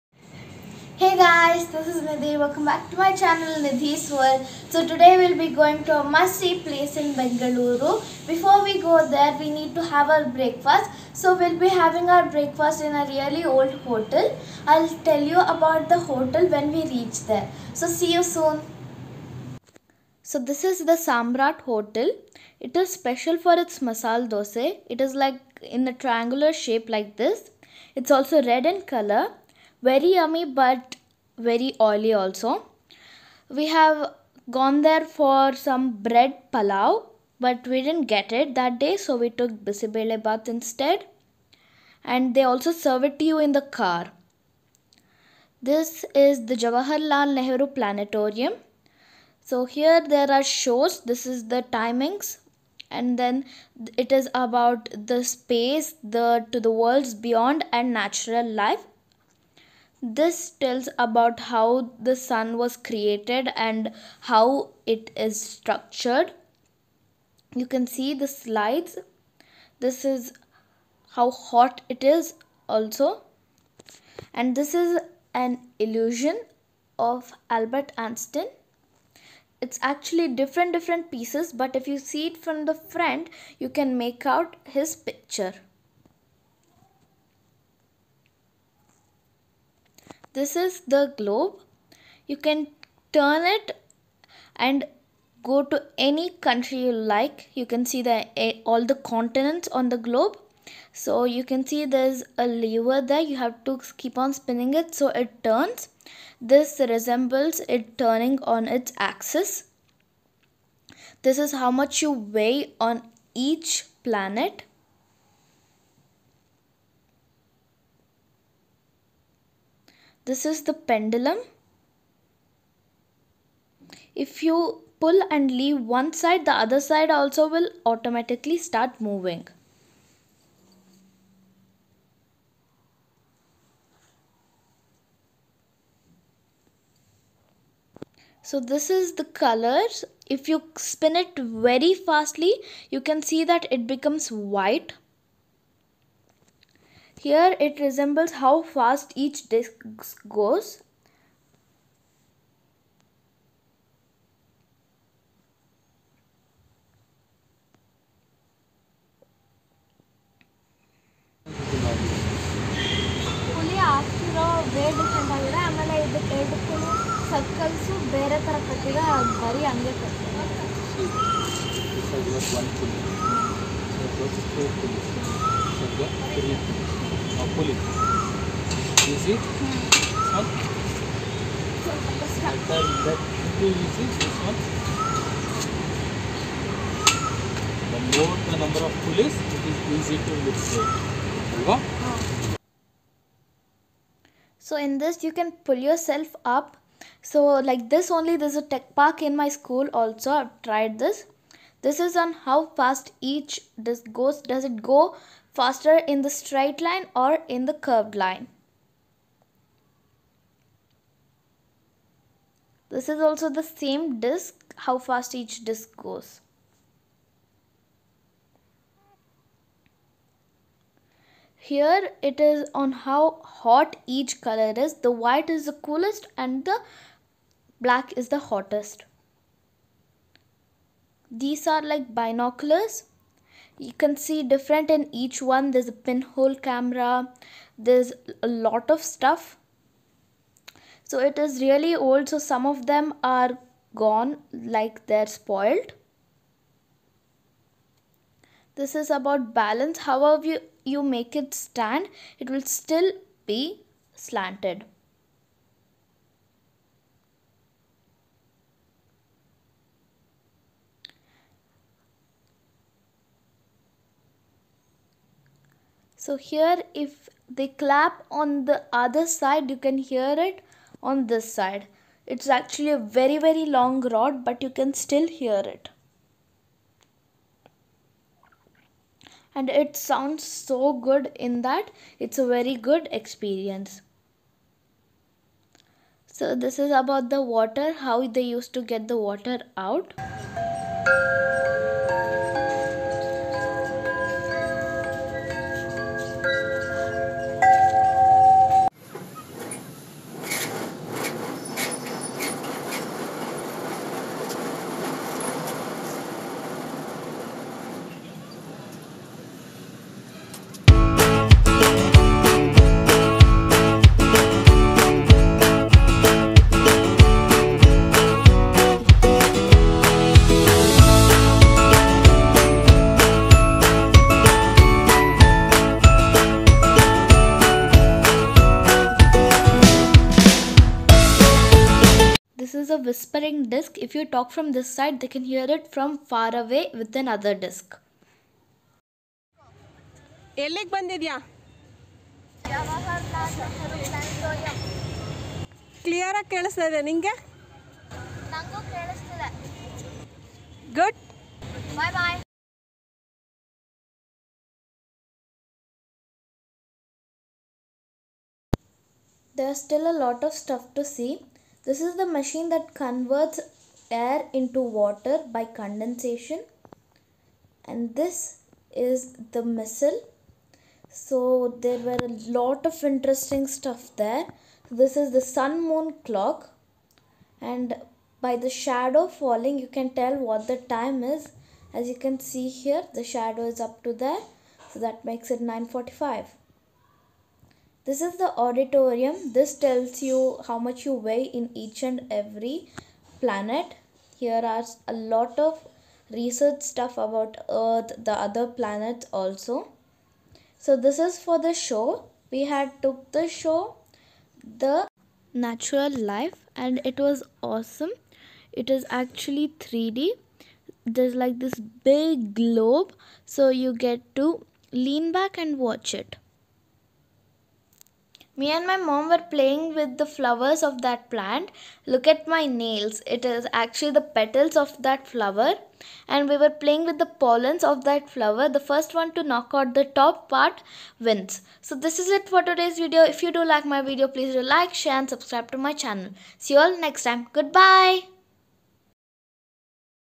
Going to Jawaharlal Nehru Planetarium
today i went to jawaharlal nehru planetarium and the experience was awesome. It was very fun exploring the tech park.

In conclusion it is an amazing place filled with fun and knowledge